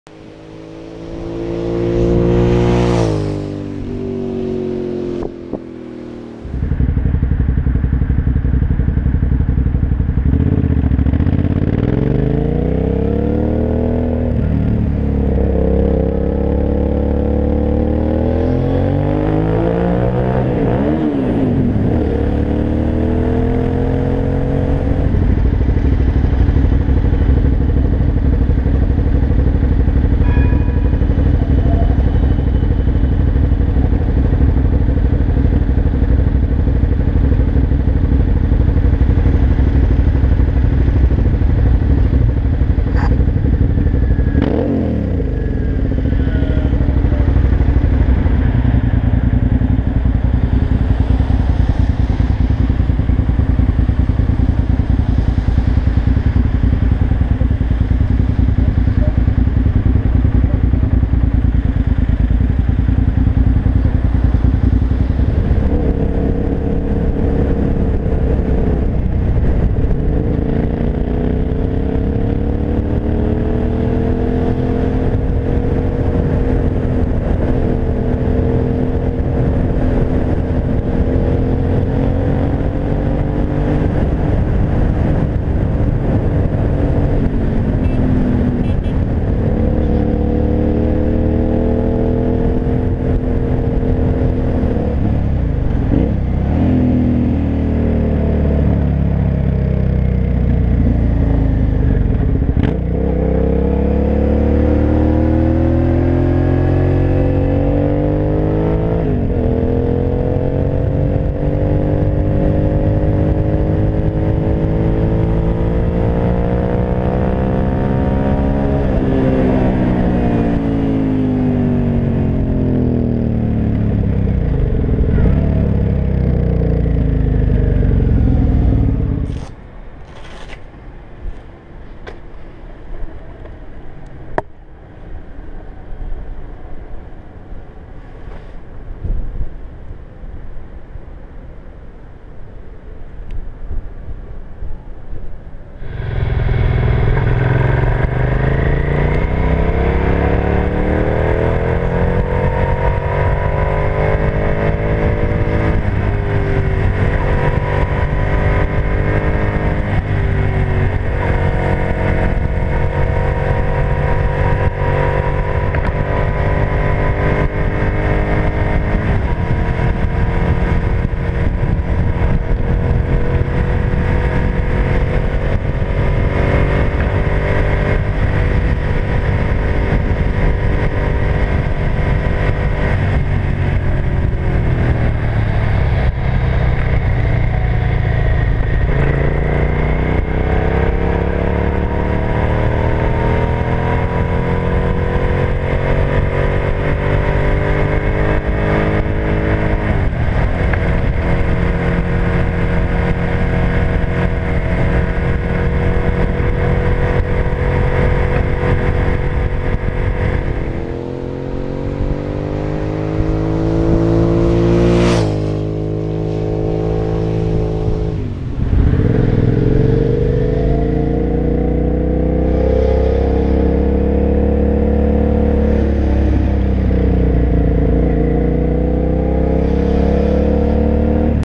Appopriate false neutral | Daily Observations 21 | Clips
Daily Observations of this video are:
Appopriate false neutral
Blip of Throttle?
Nice Car
I'm a child
'UUHHHH'
Front View

Motorcycle:
Suzuki SV 650 '01 Engine with Laser exhaust + wind